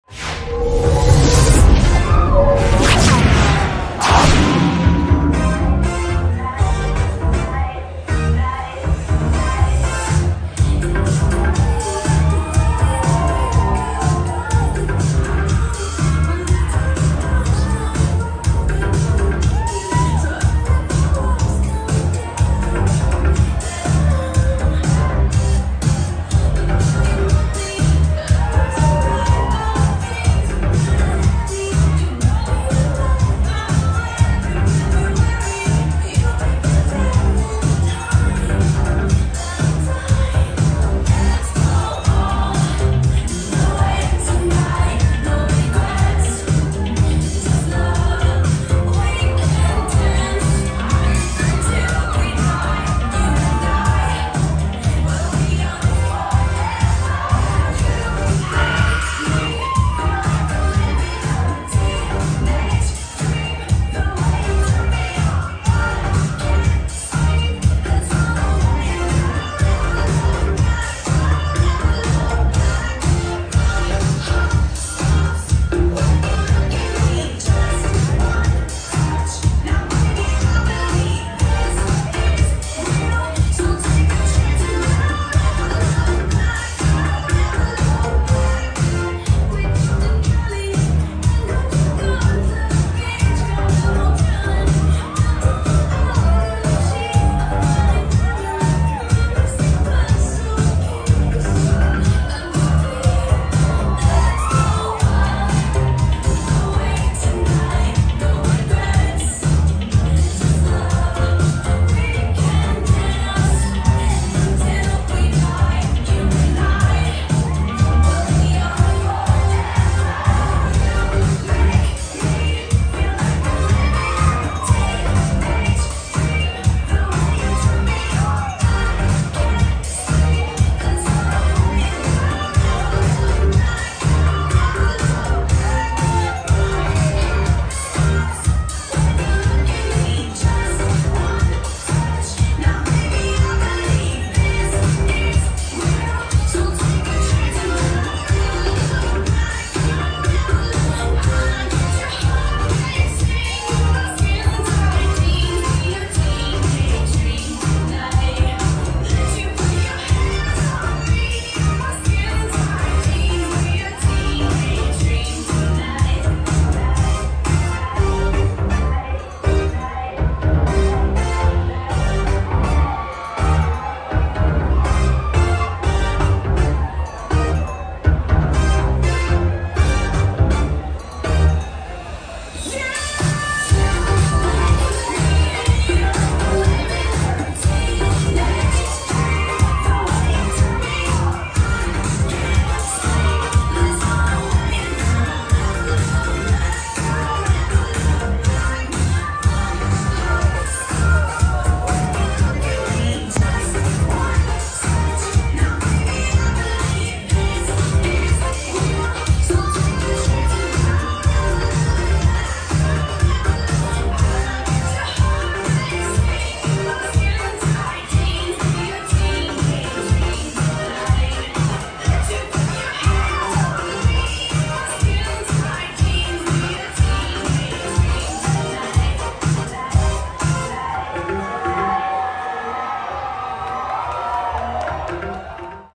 Whitney St James - Scarlet Honolulu June 17, 2022
Whitney St James slaying the legendary Pineaple Stage at  Scarlet Honolulu.

Video: Raul Rules